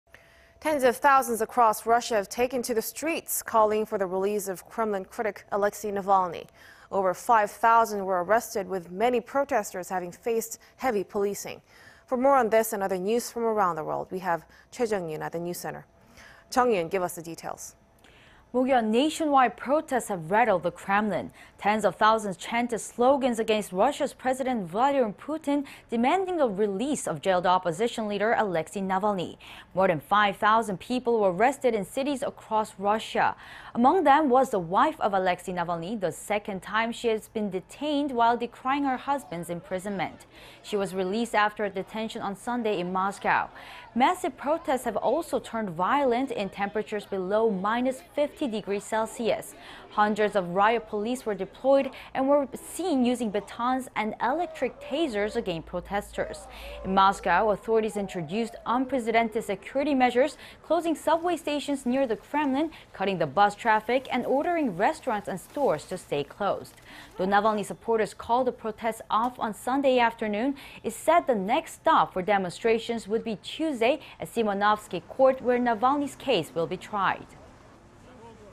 Over 5,000 detained in Russia for protests backing Navalny
러시아 전역서 나발니 지지 시위…"5천명 이상 체포"

Tens of thousands across Russia have taken to the streets calling for the release of Kremlin critic Alexei Navalny.
Over 5-thousand were arrested, with many protestors having faced heavy policing.
For more on this and other news around the world, we have Choi Jeong-yoon at the news center.
Jeong-yoon, what's the latest?
Mokyeon, nationwide protests have rattled the Kremlin.
Tens of thousands chanted slogans against Russia's President Vladimir Putin, demanding the release of jailed opposition leader Alexei Navalny.
More than 5-thousand people were arrested in cities across Russia.
Among them was the wife of Alexei Navalny the second time that she has been detained while decrying her husband’s imprisonment.
She was released after her detention on Sunday in Moscow.
Massive protests have also turned violent in temperatures below minus 50 degrees Celsius.
Hundreds of riot police were deployed, and were seen using batons and electric tasers against protesters.
In Moscow, authorities introduced unprecedented security measures, closing subway stations near the Kremlin, cutting bus traffic and ordering restaurants and stores to stay closed.
Though Navalny supporters called the protests off on Sunday afternoon, it said the "next stop" for demonstrations would be Tuesday at Simonovsky Court, where Navalny's case will be tried.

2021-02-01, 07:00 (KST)